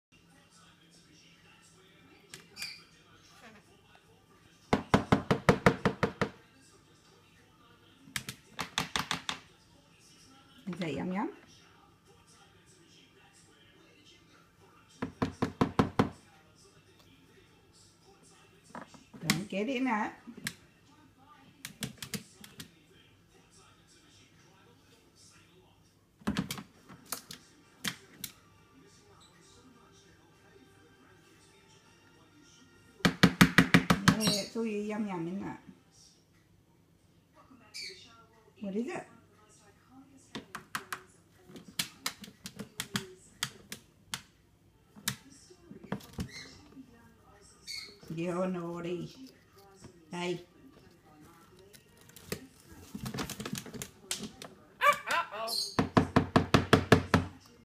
Jellybean waiting for breakfast
I was making Jellybean's chop and he wasn't being very patient. In his defense he was waiting for his breakfast.